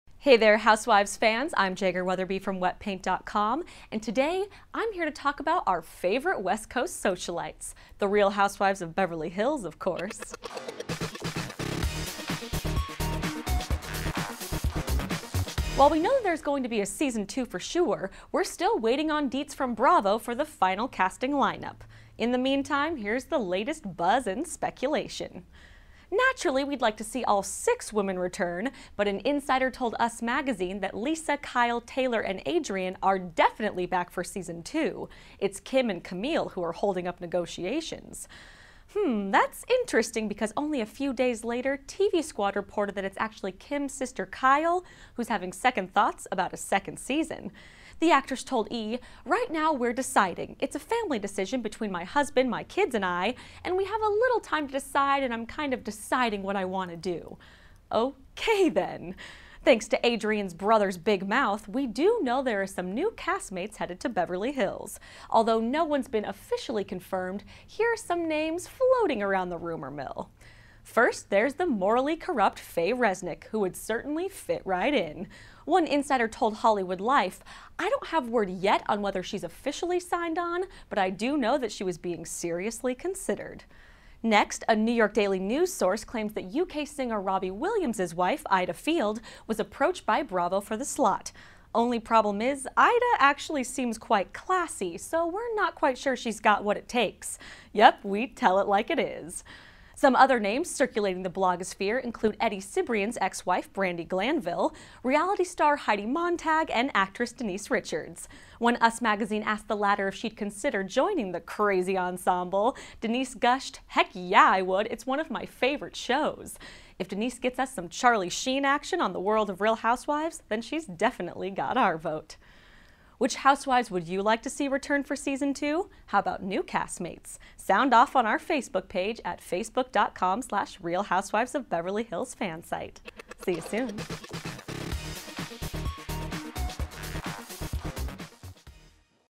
Real Housewives of Beverly Hills Season 2 Casting Buzz
- Visit us for more Real Housewives of Beverly Hills exclusives, gossip, and spoilers!

Hey there, Housewives fans! and today I'm here to talk about our favorite West Coast socialites, the Real Housewives of Beverly Hills, of course.

While we know that there's going to be a Season 2 for sure — we're still waiting on deets from Bravo for the final casting lineup. In the meantime, here's the latest buzz and speculation...

Naturally, we'd like to see all six women return, but an insider told Us Magazine that "Lisa, Kyle, Taylor, and Adrienne are definitely back for Season 2. It's Kim and Camille who are holding up negotiations."

Hmm... that's interesting because only a few days later, TV Squad reported that it's actually Kim's sister Kyle who's having second thoughts about a second season. The actress told E!, "Right now we're deciding. It's a family decision between my husband, my kids and I, and we have a little time to decide and I'm kind of deciding what I want to do." Ok, then.

Thanks to Adrienne's brother's big mouth, we do know that there are some new castmates headed to Beverly Hills. Although no one's been officially confirmed, here are some names floating around the rumor mill.

First, there's the "morally corrupt" Faye Resnick, who would certainly fit right in. One insider told Hollywood Life, "I don't have word yet on whether she has officially signed on, but I do know that she was being seriously considered."

Next, a New York Daily News source claims that UK singer Robbie Williams' wife, Ayda Field, was approached by Bravo for the slot. Only problem is, Ayda actually seems quite classy, so we're not sure she's got what it takes. Yep, we tell it like it is!

Some other names circulating the blogosphere include Eddie Cibrian's ex-wife Brandi Glanville, reality star Heidi Montag, and actress Denise Richards. When Us Magazine asked the latter if she'd consider joining the crazy ensemble, Denise gushed, "Heck yeah, I would! It's one of my favorite shows." If Denise gets us some Charlie Sheen on the world of Real Housewives, then she's definitely got our vote.

Which Housewives would you like to see return for Season 2? How about new castmates? See ya soon!